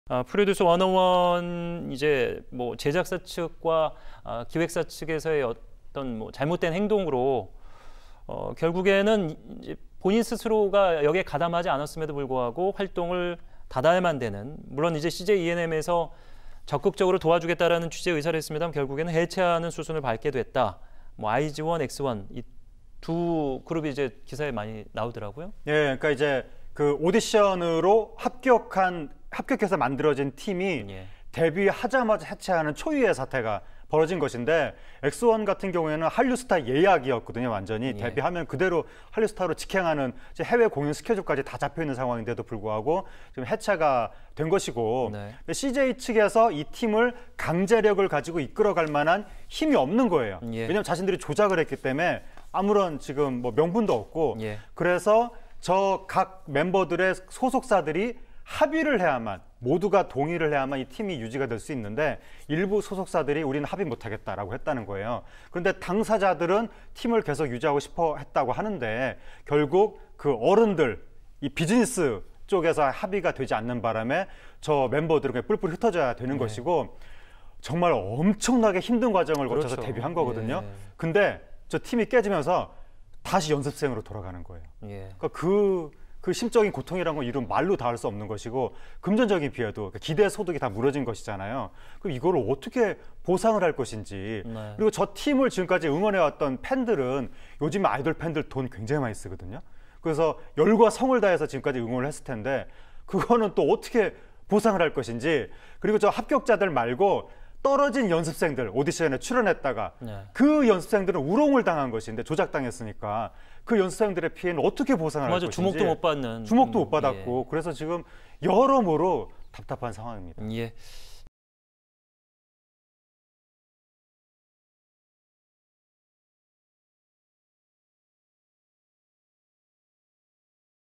엑스원 데뷔 4개월 만에 해체 / “짧은 시간 꿈같았다” 엑스원 멤버들 심경 고백 / 팬들은 활동 요구
엠넷의 아이돌 오디션 프로그램 ‘프로듀스 엑스 101’을 통해 최종 데뷔한 그룹 엑스원이 투표조작 논란으로 결국 해체하기로 했습니다. 자세한 내용 뉴스오늘에서 짚어봤습니다.